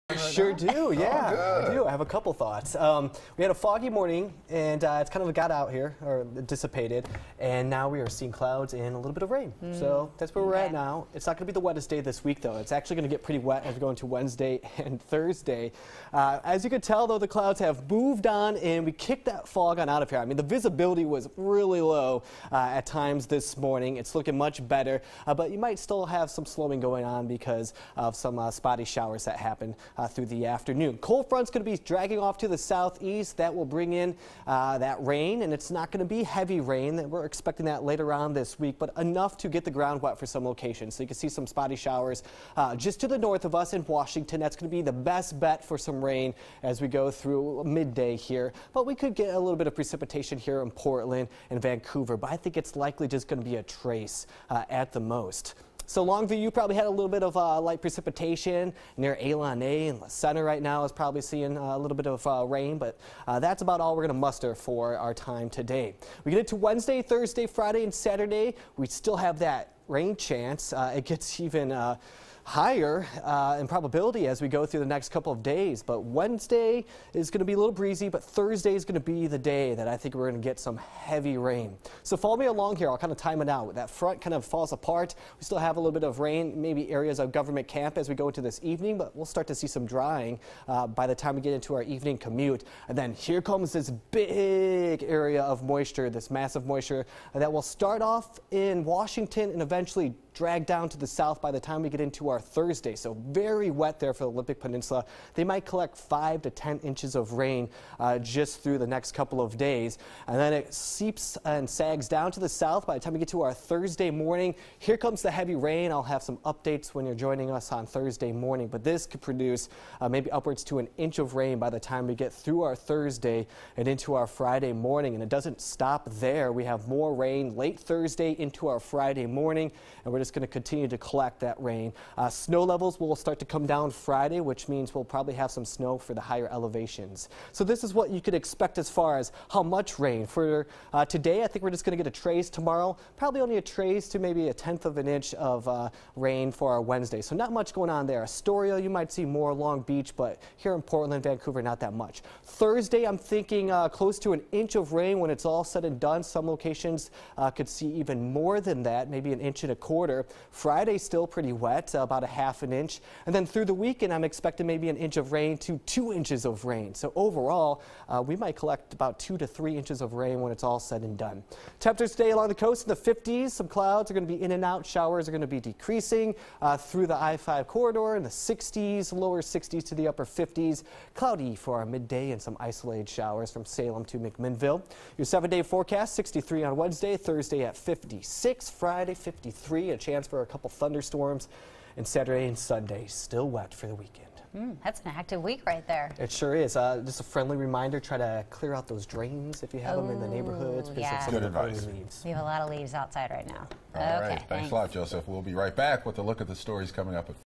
Tuesday Noon Weather Update October 17th, 2017
A few showers today, Joseph has your weather update